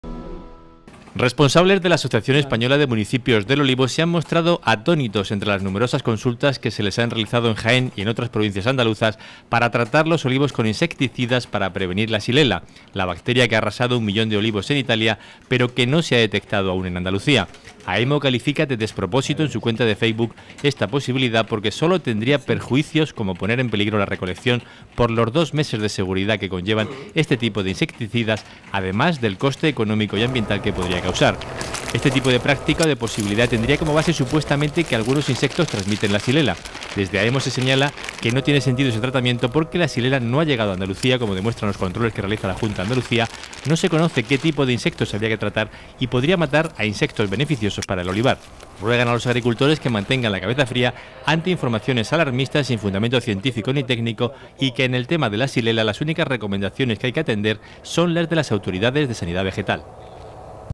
Los municipios del olivo califican de despropósito el uso de insecticidas para prevenir la xylella
La Asociación Española de Municipios del Olivo se han mostrado atónitos ante las numerosas consultas que se les han realizado en Jaén, y en otras provincias andaluzas, para tratar los olivos con insecticidas para prevenir la xylella, la bacteria que ha arrasado un millón de olivos en Italia pero que no se ha detectado en Andalucía. Aemo califica de despropósito, en su cuenta de facebook, esta posibilidad  porque sólo tendría perjuicios como poner en peligro la recolección por los dos meses de seguridad que conllevan este tipo de insecticidas, además del coste económico y ambiental que podría causar.